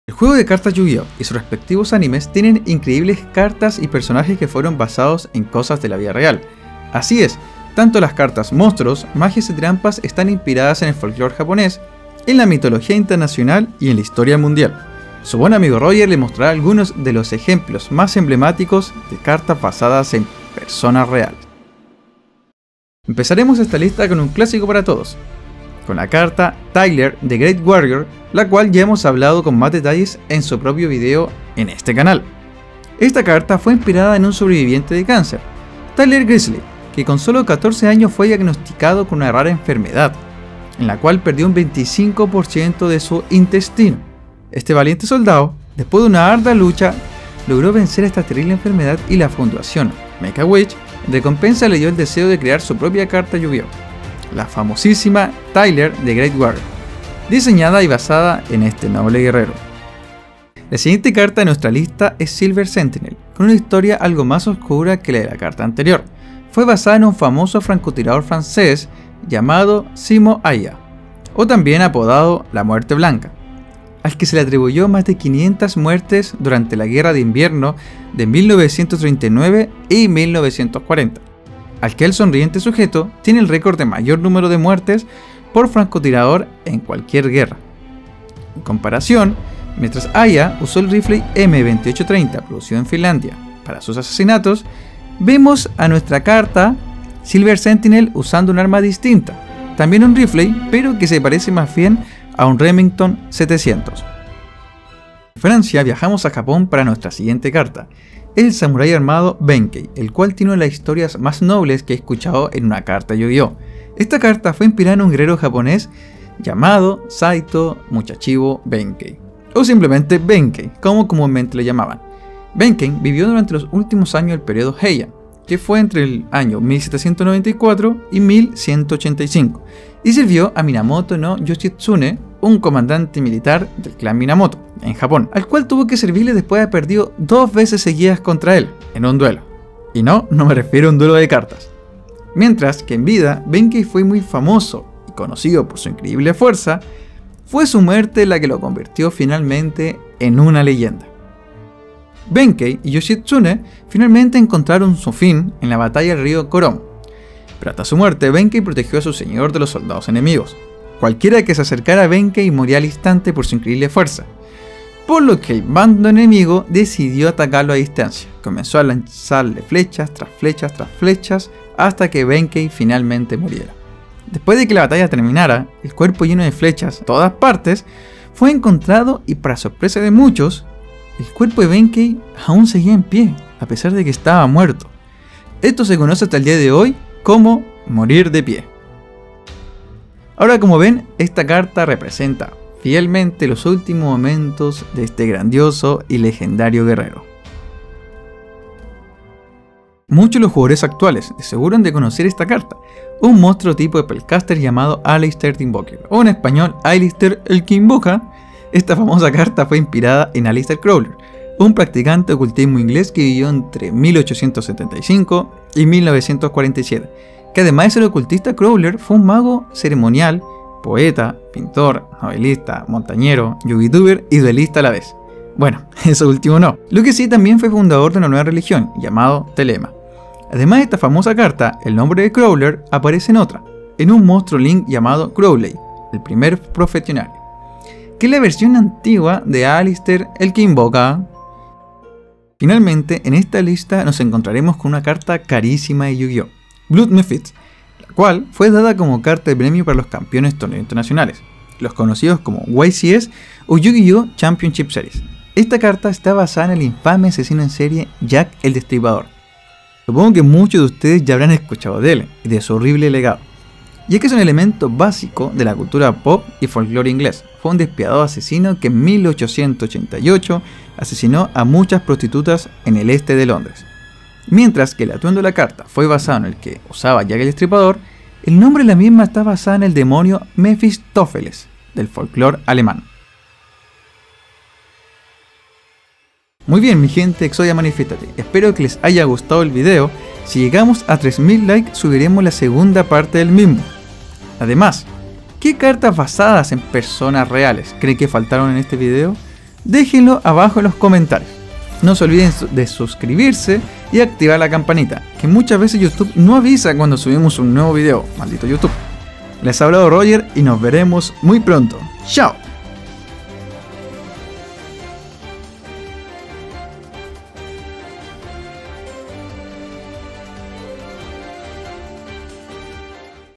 CARTAS BASADAS EN PERSONAS REALES
Muchas cartas de Yu-gi-oh, están basadas o inspiradas en personas o historias reales, en este video se presentan varios ejemplos: tyler the great warrior, jack el destripador, Benkei, Aleister Crowler, entre muchos otros personajes históricos!